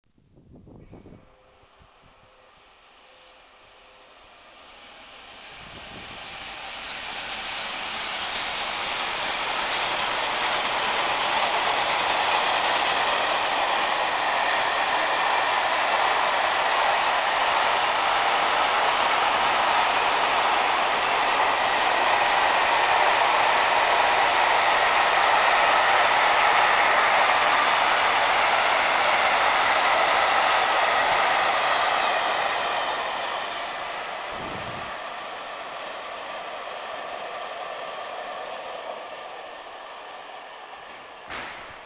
GZ nach Nü.ASF
Güterzug mit 2x BR 185 nach Nürnberg, durch Oberhaid.